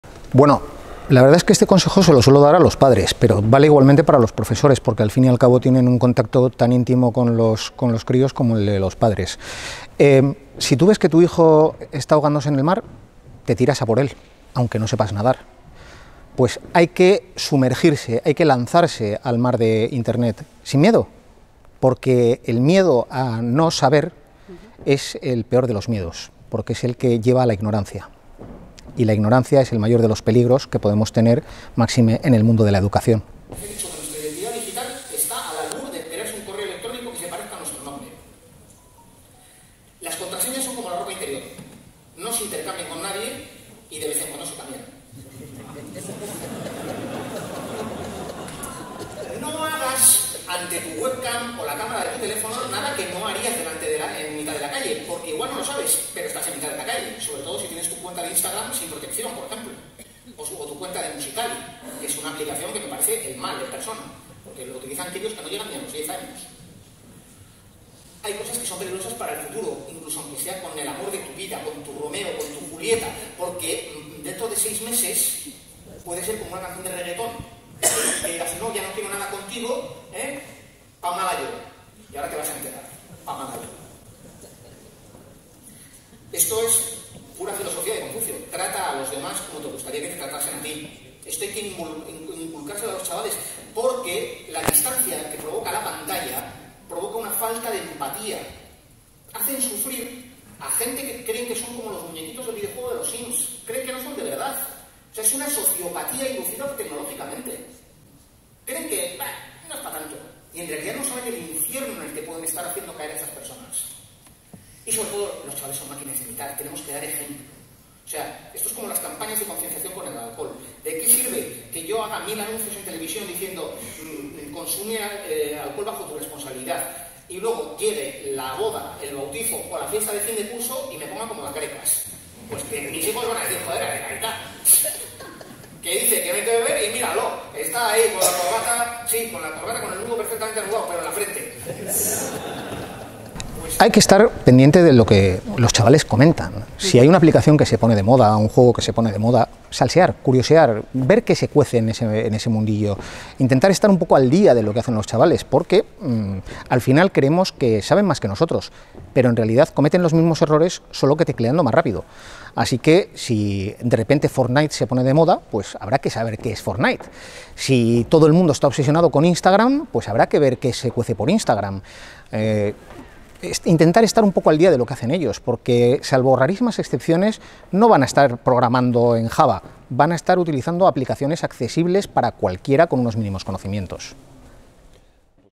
videotwit con Jorge Bermudez en 15Adide
En 15Adide Jorge Bermúdez, Fiscal Delegado de Delitos Informáticos en Gipuzkoa, da unos conesjos para la Gestión de la convivencia en la era digital, en el congreso de @AdideFederacion. JosiIKT - @BGNagusia CCbySA